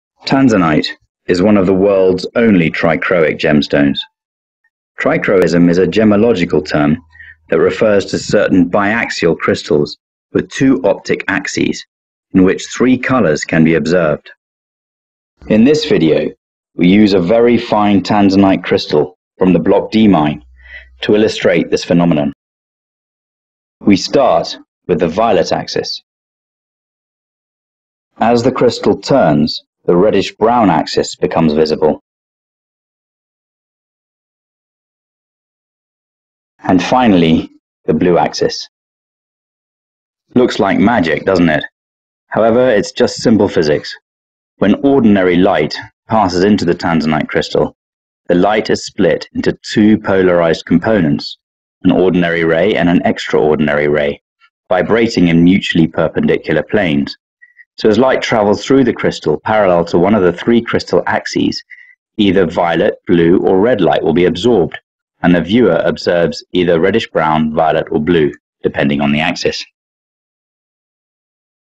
What is Trichroism in Tanzanite?
- This video explains Tanzanite's extraordinary and unique phenomenon of Trichroism - Learn what Tanzanite Trichrosim is and how this affects Tanzanite's color.

Tanzanite is the world's only Trichroic gemstone. Trichroism is a gemological term that refers to bi axial gem crystals with two optic axis in which 3 colors can be observed - Violet, reddish brown and blue.

A case of simple science where light shining through the Tanzanite crystal is split into 2
different angles and reflects light back to the viewer as different color depending
on the angel of the crystal.

This is what make Tanzanite so unique and why you hear the term "Red Flash" in Tanzanite.